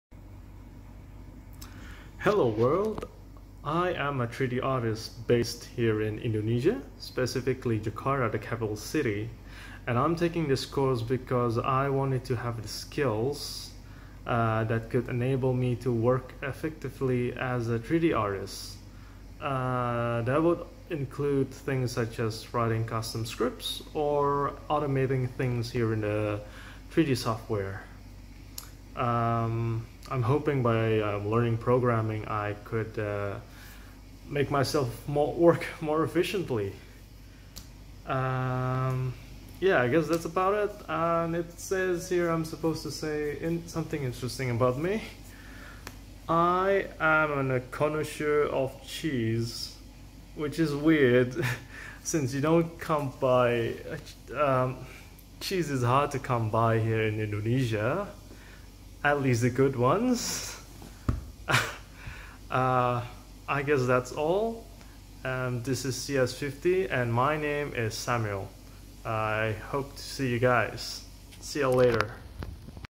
CS50 Intro Vid - Samuel C.A
Hello World,
This is my intro video to Harvard's CS50 computer science course.
I hope to learn from you all!